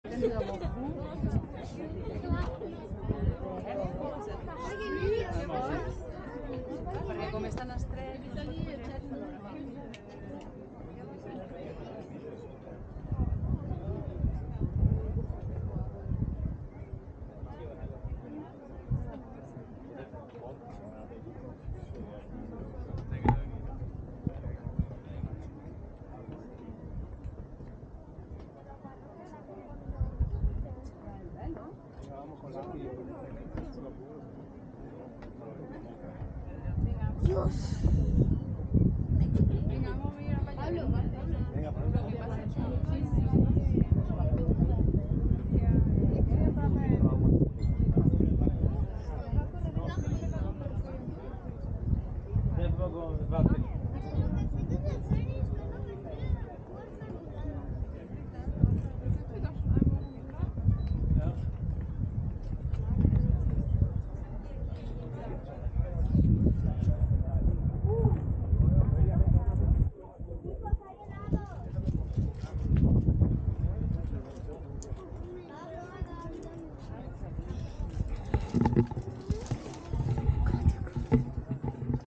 Aussicht vom Runden Turm in Kopenhagen über die Stadt
Während unseres Sommerurlaubs in Dänemark haben wir als Familie auch zwei Tage in der Hauptstadt Kopenhagen verbracht. Als Sehenswürdigkeit wurde mir auf Twitter insbesondere der Runde Turm (dänisch: Rundetårn) in der Innenstadt empfohlen.

Ein Besuch lohnt sich auf jeden Fall, die Aussicht von der Plattform ist phänomenal, man blickt über die gesamte Stadt. Der Aufstieg verläuft quasi ohne Treppen, weil der Turm durch einen gemauerten Spiralgang verfügt, über den man nach oben läuft.